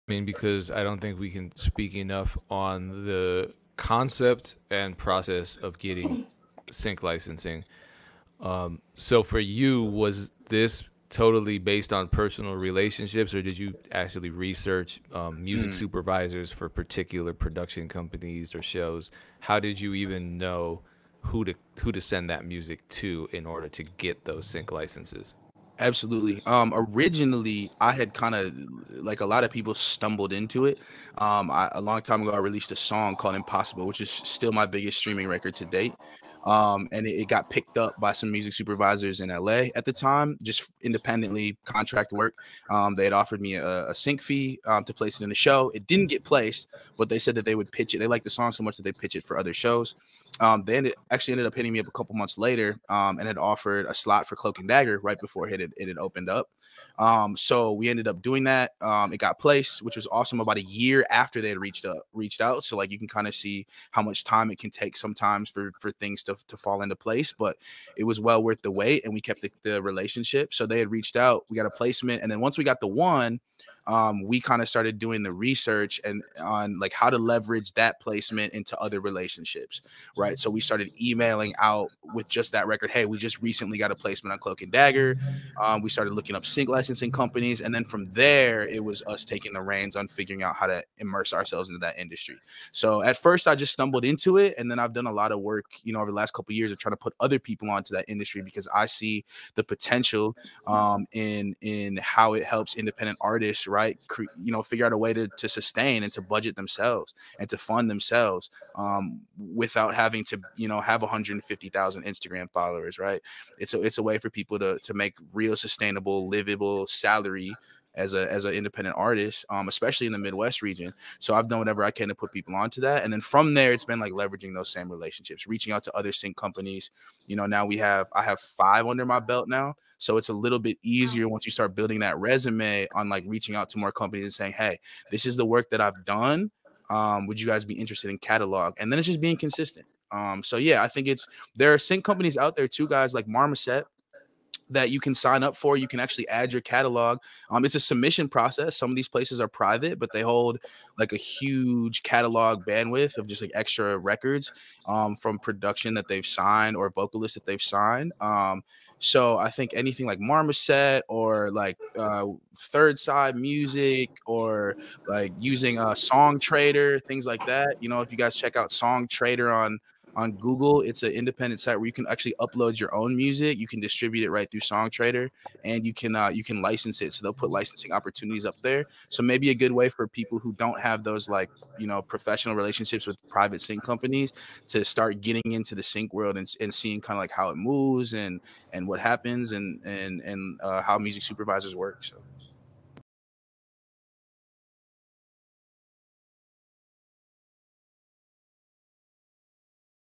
How to Get My Beats on TV
Explore the world of sync licensing and learn how you can get your beats on TV like Lucien Parker did! Also, remember to tune for more content like this every Thursday at 3PM EST for the MEC Show with DJ Pain 1.